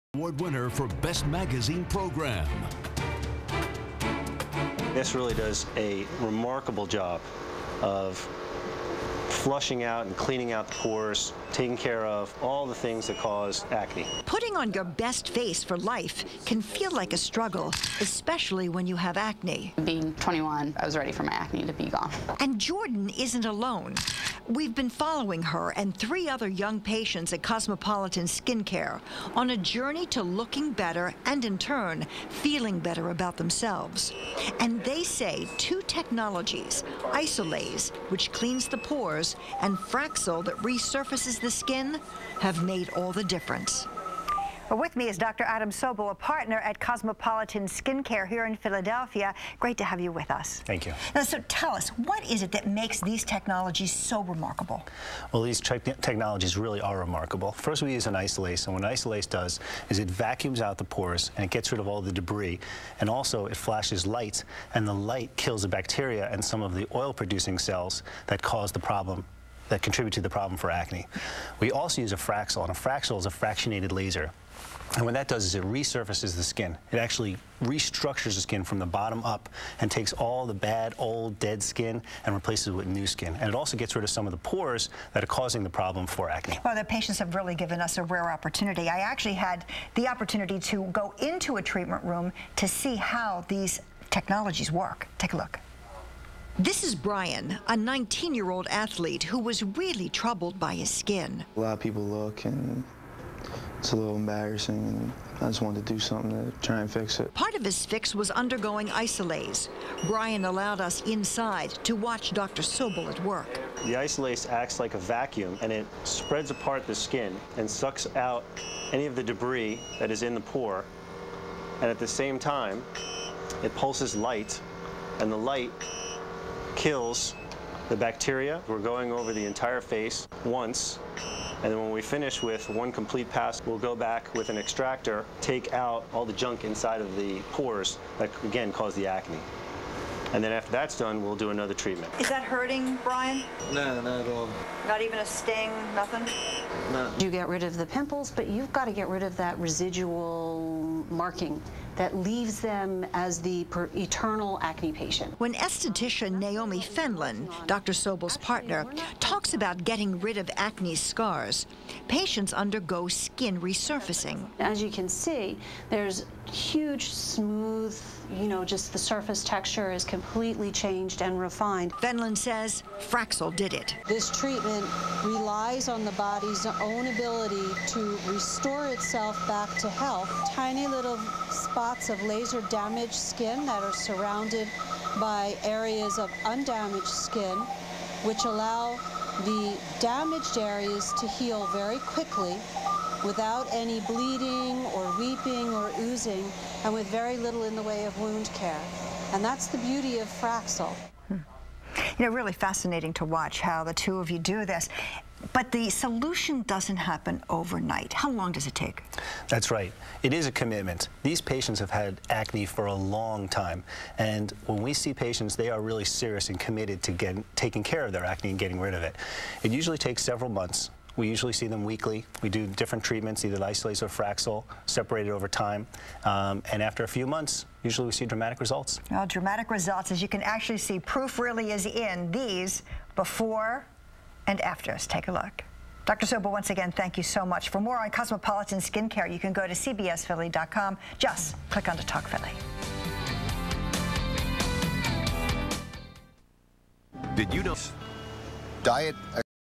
What are the best laser acne treatments?
Philadelphia based Naomi Fenlin has developed a unique way to permanently get rid of acne-- even the most persistent cases. Fraxel and Isolaz laser treatments enhance the functioning and appearance of the skin.